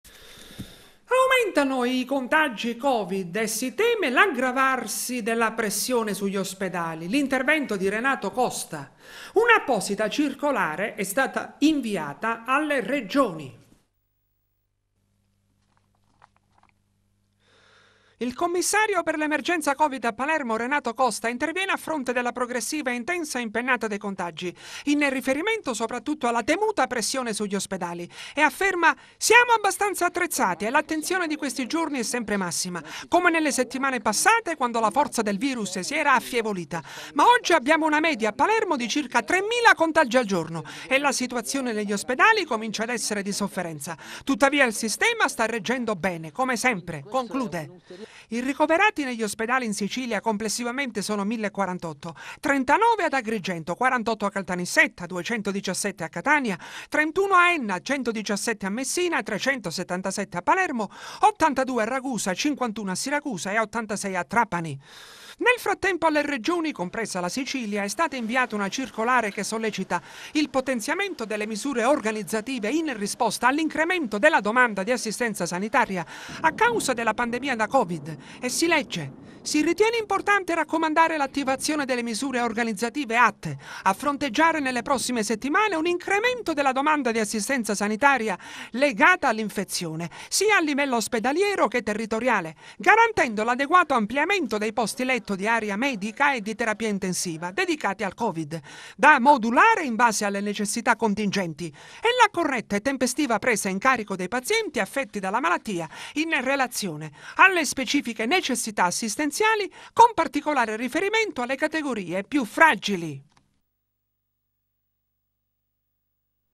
Teleacras - Il covid e la pressione sugli ospedali
Aumentano i contagi covid e si teme l’aggravarsi della pressione sugli ospedali. L’intervento di Renato Costa. Un’apposita circolare è stata inviata alle Regioni. Il servizio di Angelo Ruoppolo.

Il commissario per l’emergenza covid a Palermo, Renato Costa, interviene a fronte della progressiva e intensa impennata dei contagi, in riferimento soprattutto alla temuta pressione sugli ospedali. E afferma: “Siamo abbastanza attrezzati e l'attenzione di questi giorni è sempre massima, come nelle settimane passate quando la forza del virus si era affievolita, ma oggi abbiamo una media a Palermo di circa 3mila contagi al giorno e la situazione negli ospedali comincia ad essere di sofferenza, tuttavia il sistema sta reggendo bene, come sempre” – conclude. I ricoverati negli ospedali in Sicilia complessivamente sono 1.048: 39 ad Agrigento, 48 a Caltanissetta, 217 a Catania, 31 a Enna, 117 a Messina, 377 a Palermo, 82 a Ragusa, 51 a Siracusa, e 86 a Trapani. Nel frattempo alle Regioni, compresa la Sicilia, è stata inviata una circolare che sollecita il potenziamento delle misure organizzative in risposta all'incremento della domanda di assistenza sanitaria a causa della pandemia da covid. E si legge: “Si ritiene importante raccomandare l'attivazione delle misure organizzative atte a fronteggiare nelle prossime settimane un incremento della domanda di assistenza sanitaria legata all'infezione, sia a livello ospedaliero che territoriale, garantendo l'adeguato ampliamento dei posti letto di area medica e di terapia intensiva dedicati al covid, da modulare in base alle necessità contingenti, e la corretta e tempestiva presa in carico dei pazienti affetti dalla malattia in relazione alle specifiche necessità assistenziali, con particolare riferimento alle categorie più fragili”.